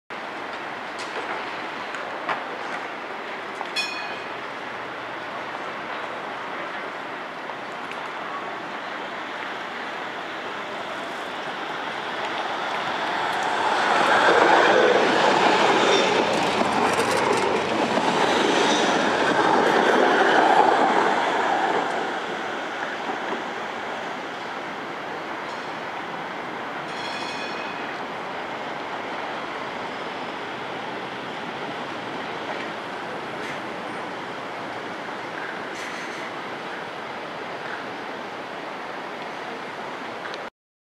Rheinbahn Straßenbahn NF10 (2033) te Düsseldorf Hbf [DE]
Rheinbahn Straßenbahn (tram) van het type NF10 vertrekt hier vanaf Düsseldorf Hauptbahnhof in de richting van het de universiteit. Dit tramtype telt maarliefst 10 assen en 7 bakken!

21 september 2012